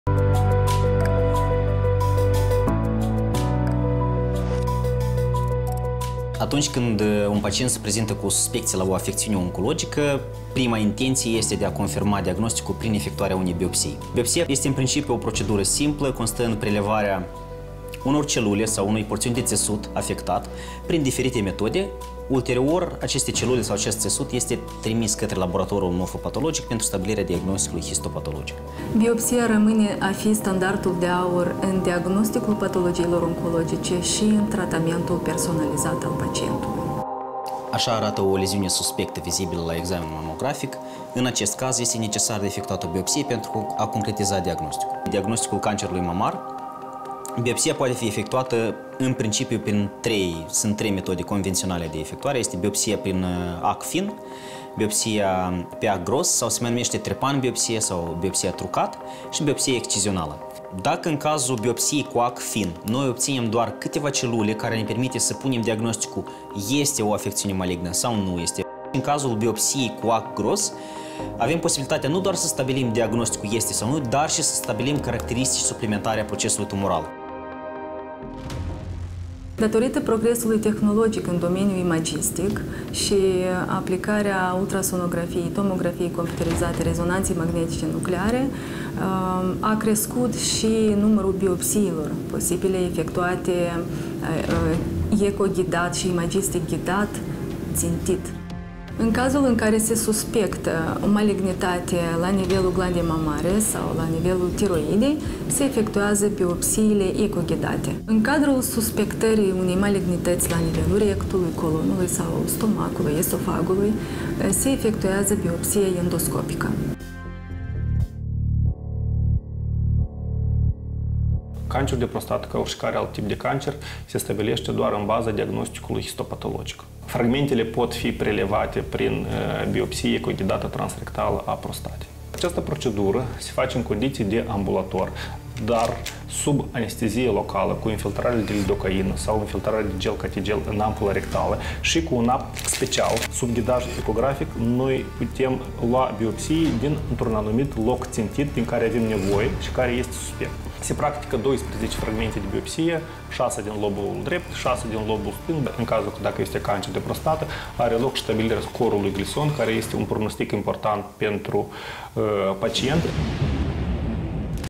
Biopsia
În diagnosticul maladiilor oncologice, unul dintre cele mai importante și informative metode  este Biopsia.